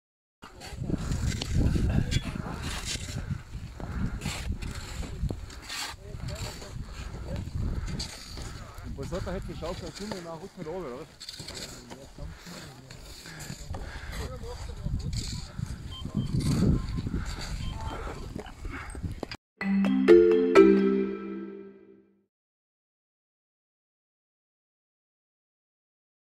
Pistenretter in Saalbach
Rund 100 kurzfristig organisierte Pistenarbeiter arbeiteten am Dienstag im Schneesturm mit Schaufeln und Fräsen, um die Weltcuprennen in Saalbach-Hinterglemm zu sichern. Da ist Improvisation gefragt. Mehr wissen [SN Plus]